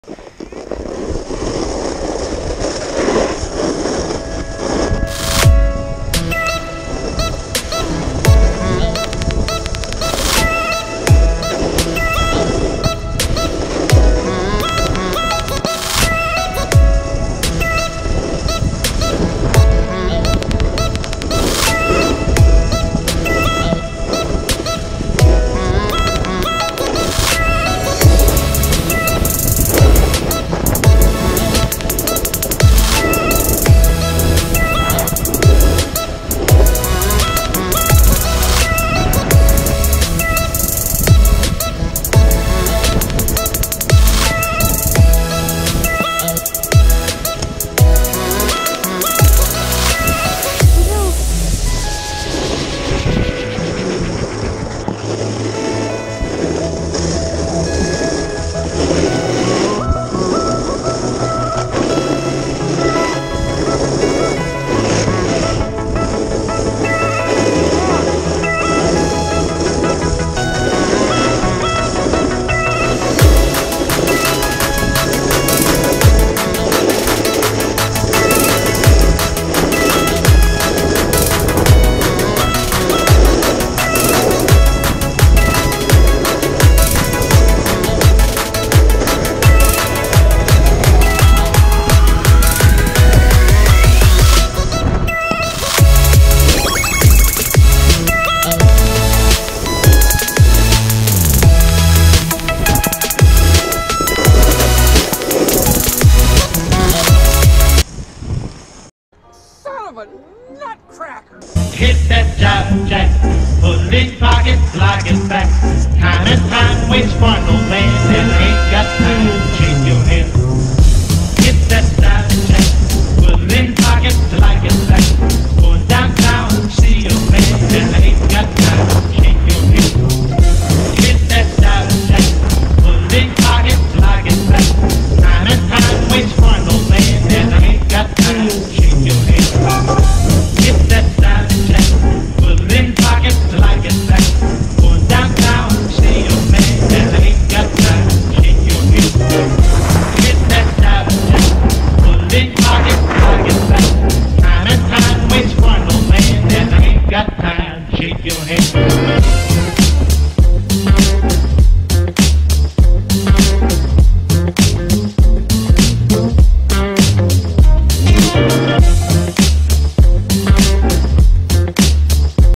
Icy day at app ski mtn
sick day riding almost every single trail there